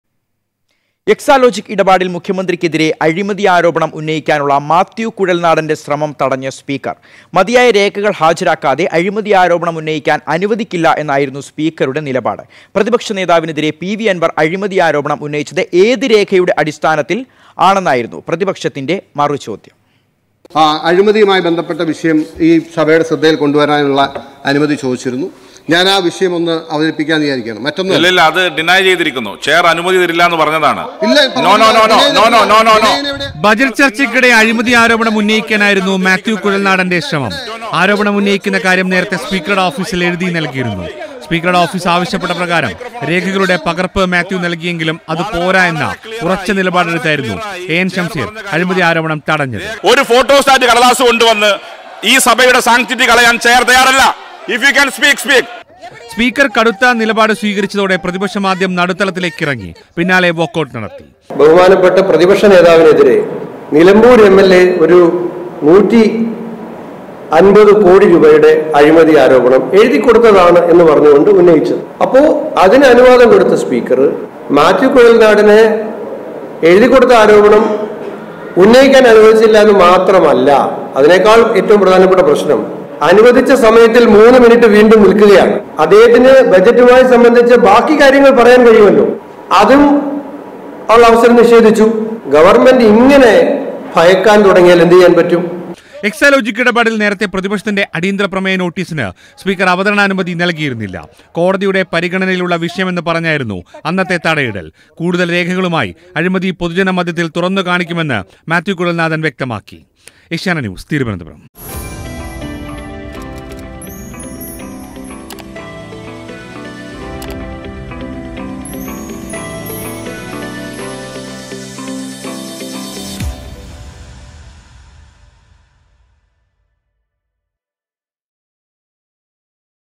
എക്സാലോജിക് ഇടപാടിൽ മാത്യു കുഴൽനാടൻ മുഖ്യമന്ത്രിക്കെതിരെ സംസാരിക്കുന്നത് തടഞ്ഞ് സ്പീക്കർ
എക്സാലോജിക് ഇടപാടിൽ മുഖ്യമന്ത്രിക്കെതിരെ അഴിമതി ആരോപണം ഉന്നയിക്കാനുള്ള മാത്യു കുഴൽനാടന്റെ ശ്രമം തടഞ്ഞ് സ്പീക്കർ; മതിയായ രേഖകൾ ഹാജരാക്കാതെ ആരോപണം ഉന്നയിക്കാനാകില്ലെന്ന് സ്പീക്കർ